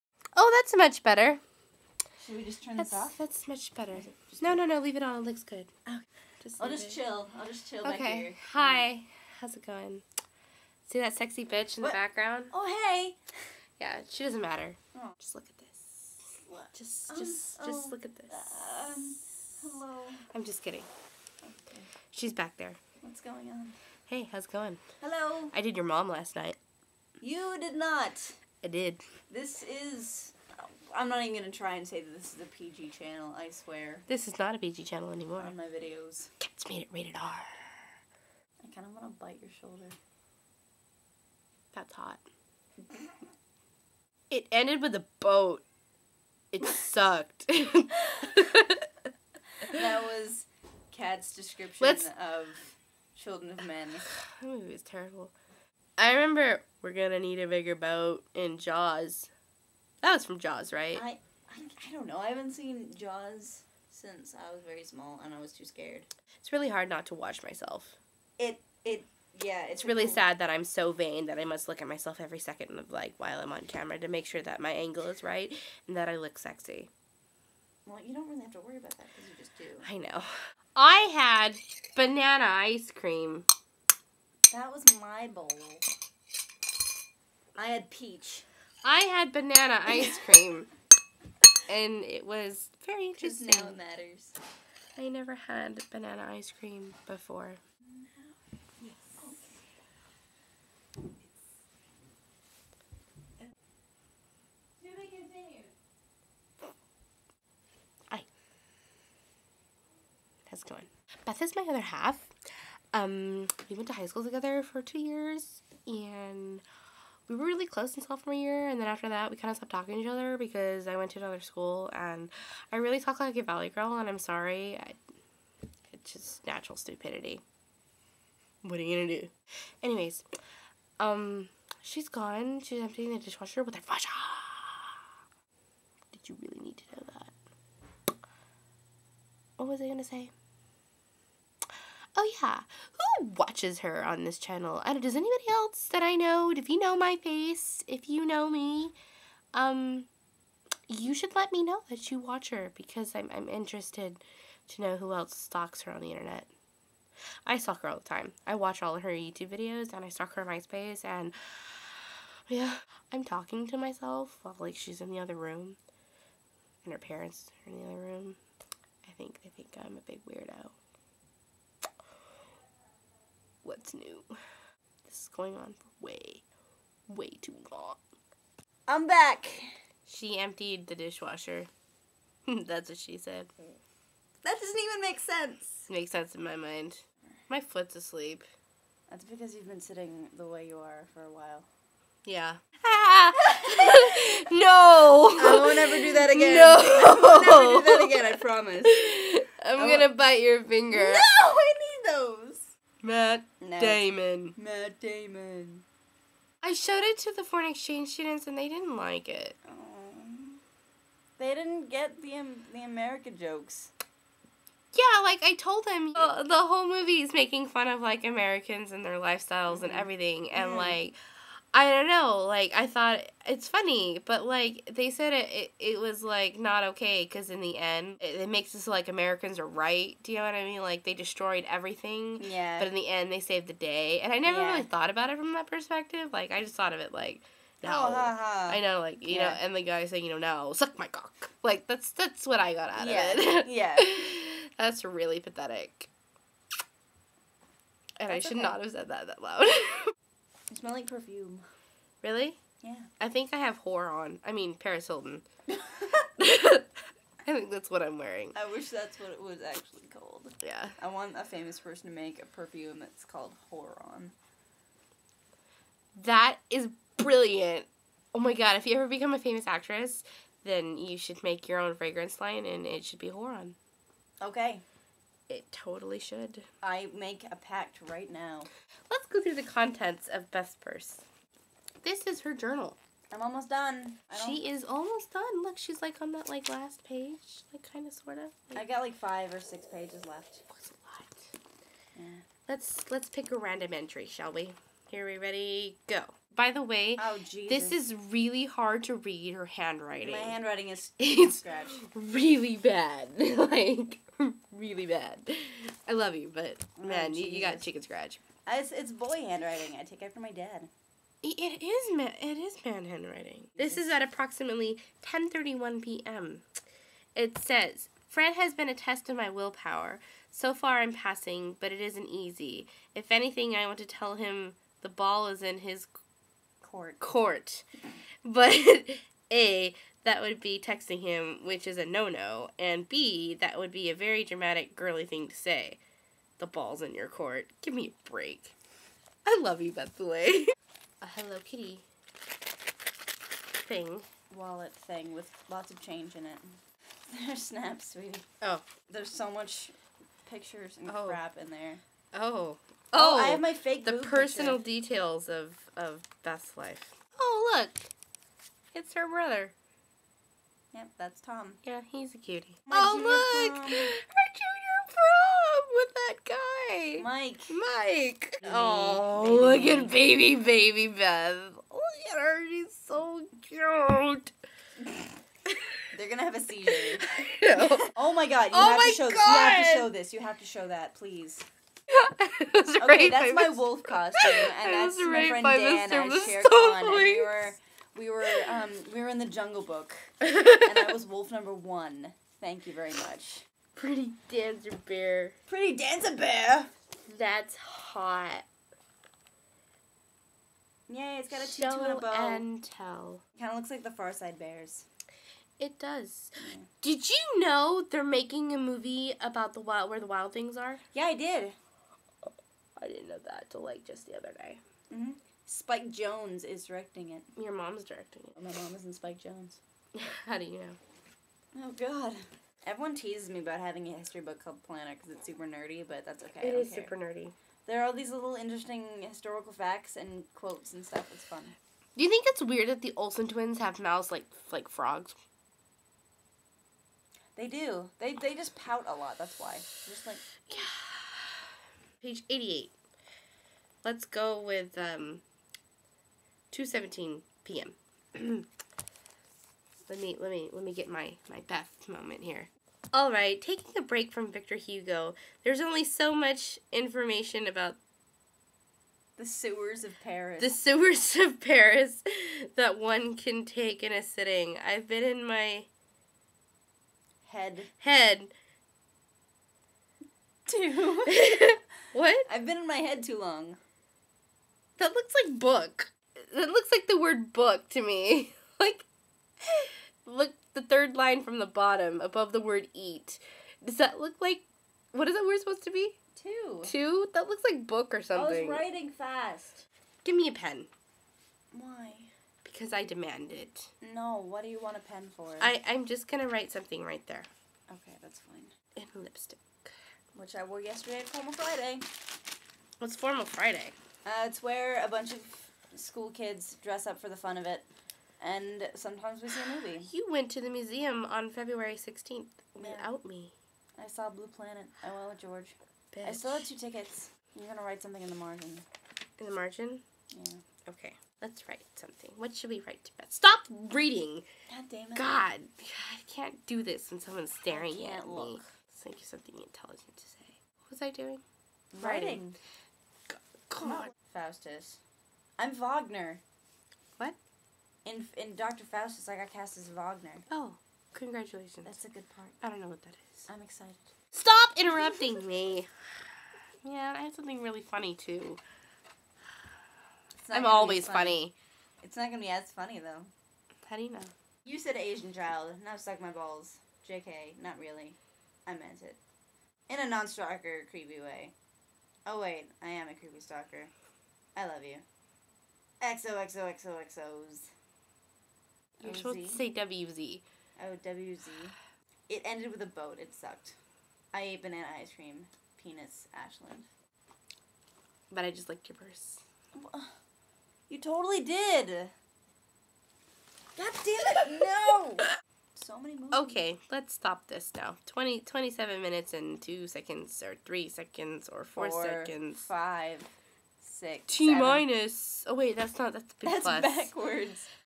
Fun times between halves
Beth and Cat hang out, talk about movies, go through Beth's purse, and are generally quite silly.